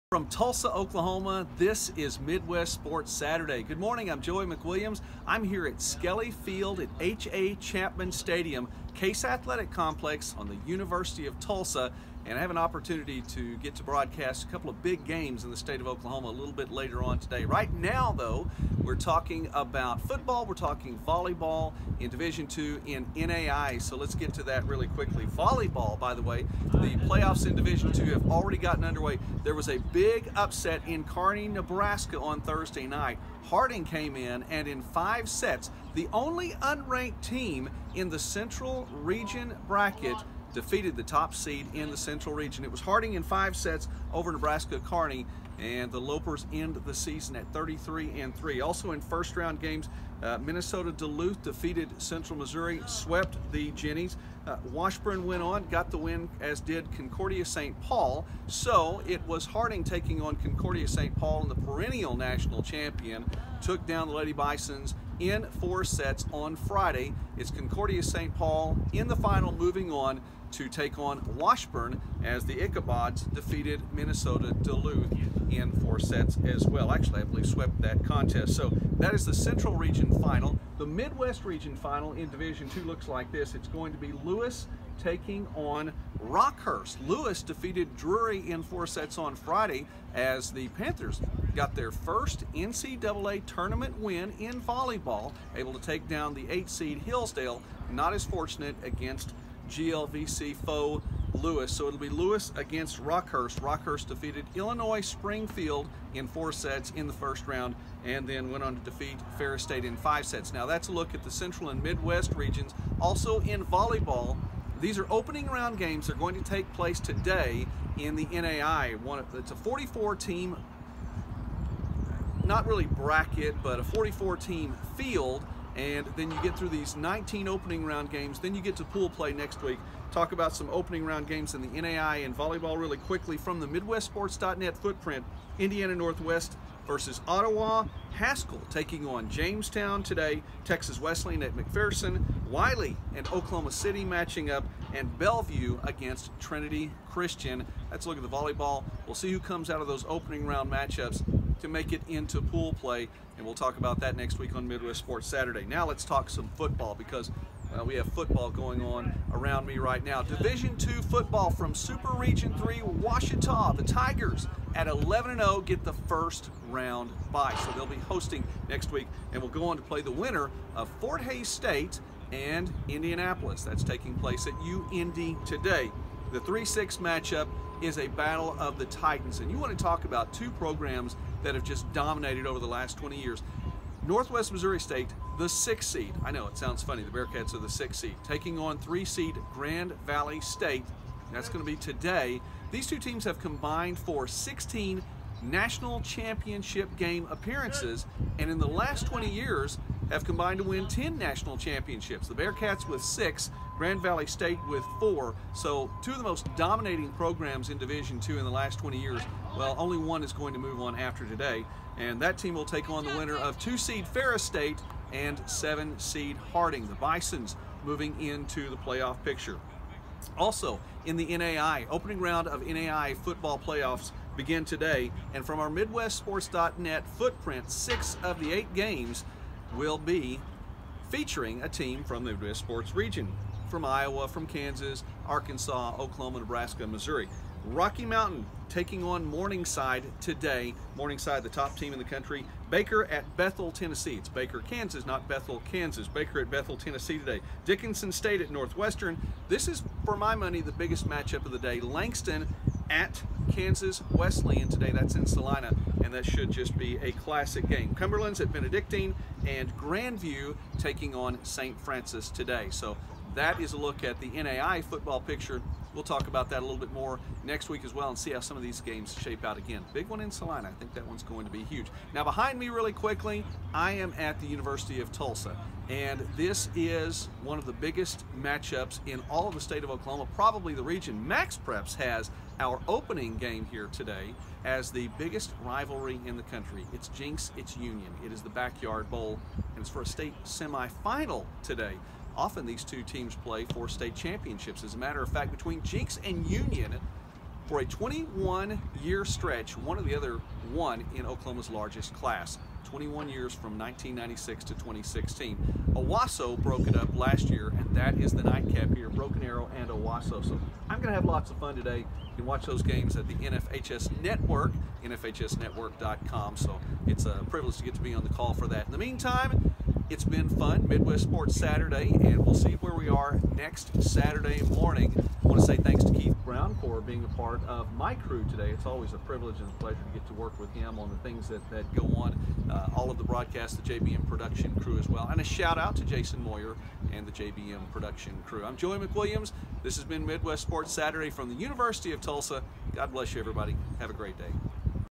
Midwest Sports Saturday 2018 - Tulsa
MSS No. 12 - From the University of Tulsa, prior to the start of the Jenks/Union football game, an abbreviated version of Midwest Sports Saturday.